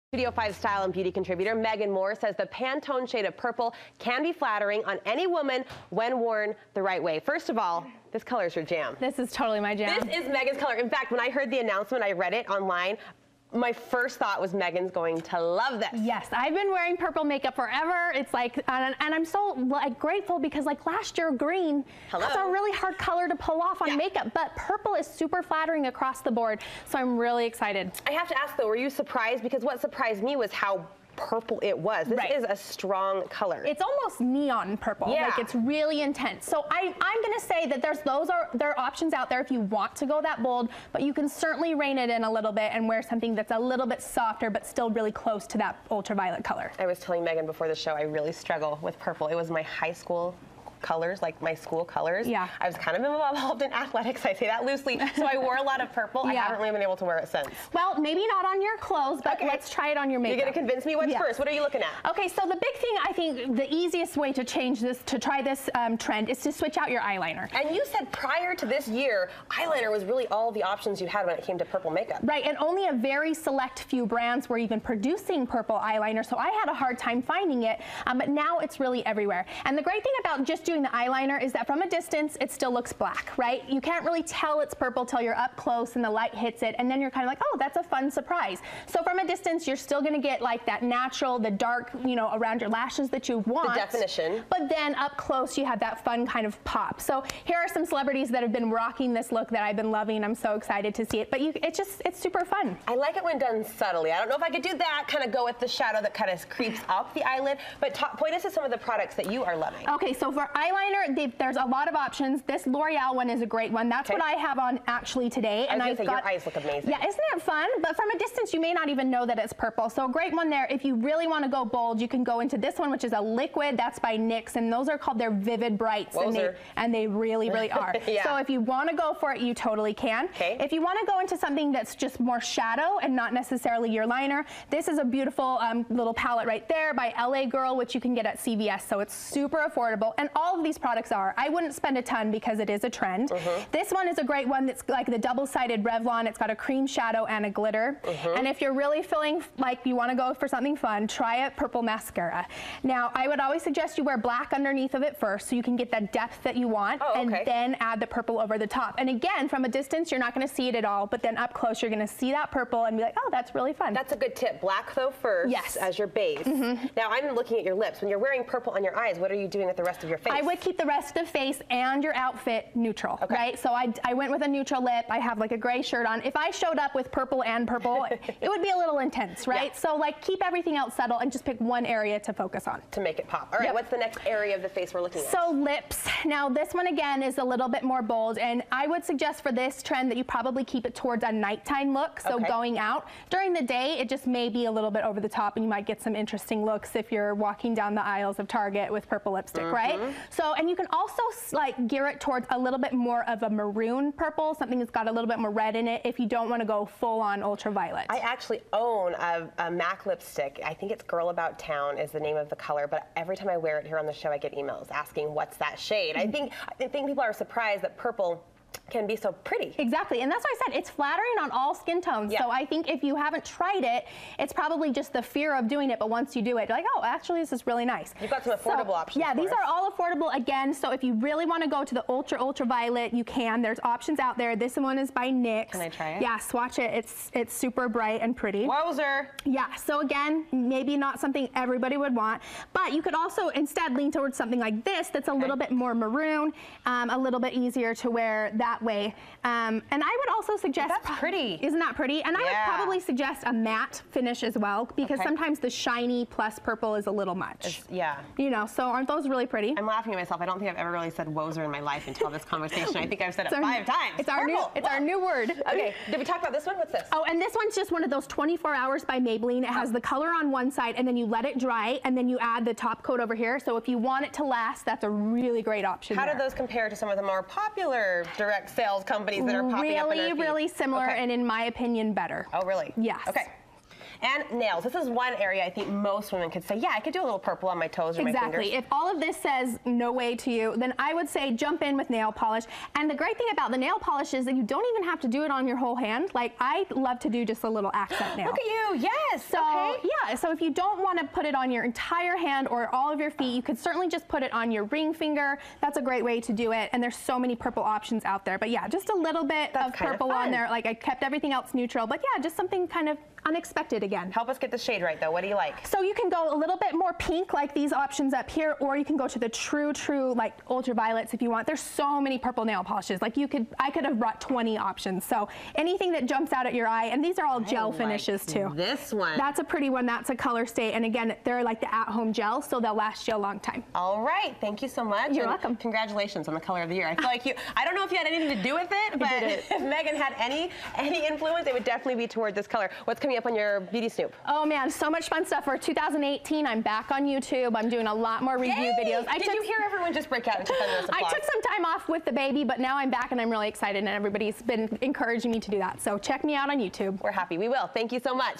Practical Ways to Add Ultra Violet Into Your Makeup Bag
ORIGINALLY AIRED JANUARY 2018 Purple might seem like a bold makeup choice, and it can be. But do it right and you'll end up with a flawlessly trendy look.

Megan Moore shares how you can pull off the color of the year in your makeup routine.